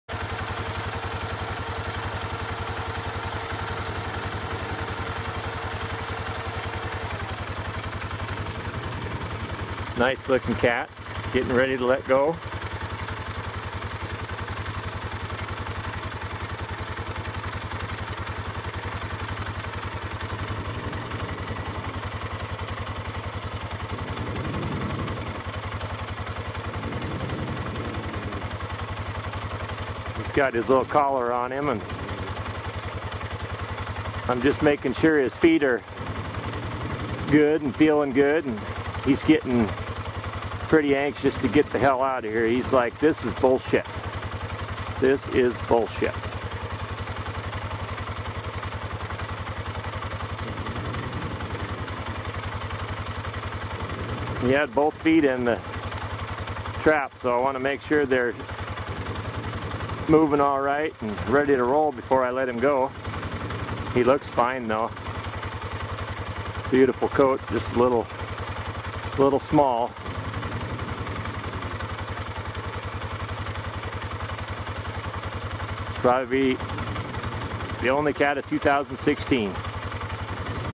Montana bobcat release
This video is of a small Bobcat released 2016. Small Bobcat. Quota filled and cat was small so he was released. Great thing about foothold traps is they can be released. Unharmed.